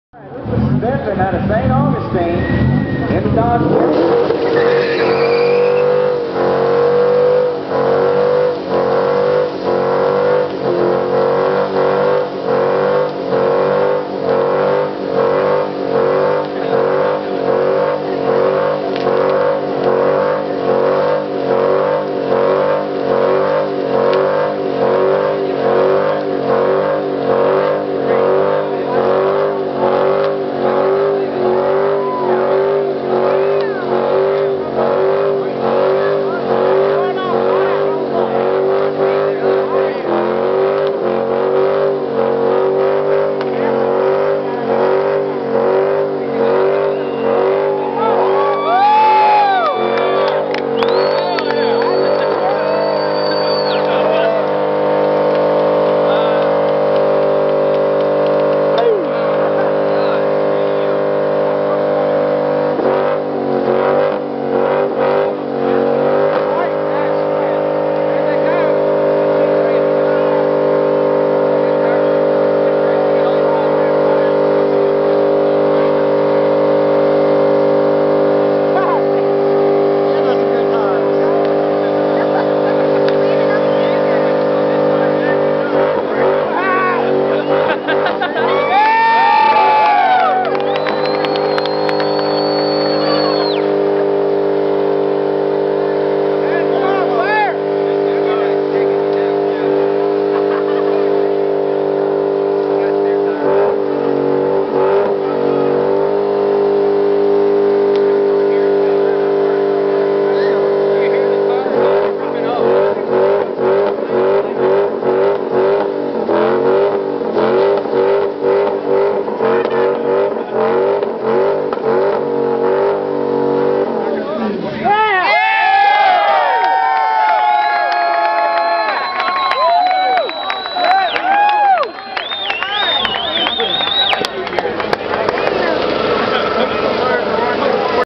Dodge challenger R/T burnout contest winner IFO 2010
a Challenger R/T's winning burnout at gainesville raceway (IFO 2010)